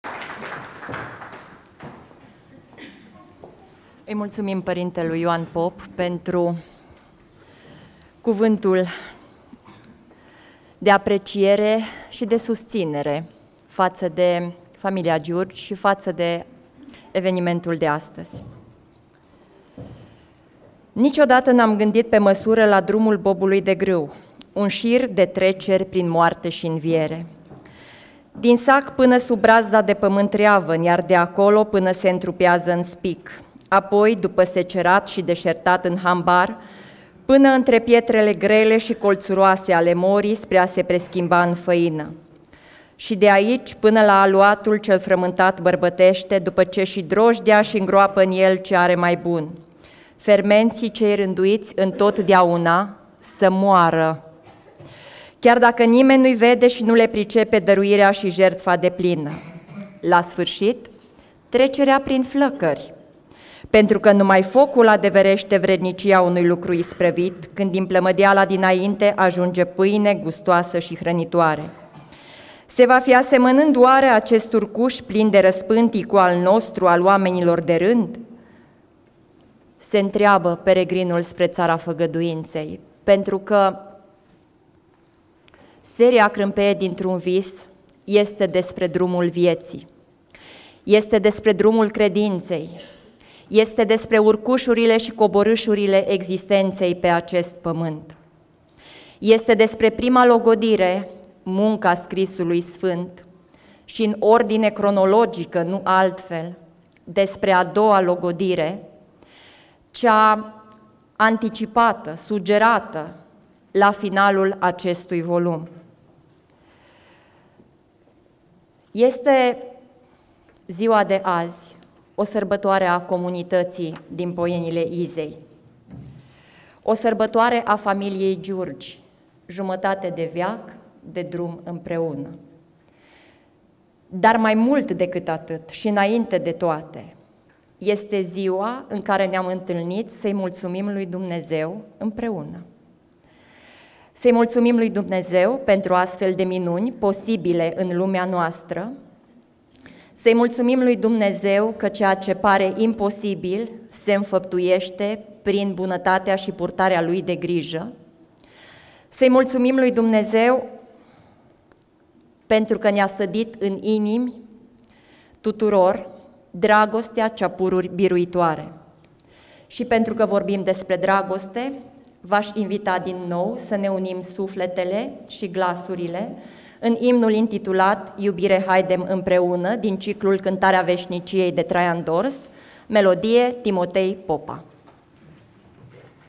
Florina-Maria Băcilă - Volumul „Crâmpeie dintr-un vis", o minune (Poienile Izei, 26 oct. 2019)
Festivitatea lansării celui de-al doilea volum „Crâmpeie dintr-un vis" semnat Petru Giurgi (Poienile Izei, 26 oct. 2019)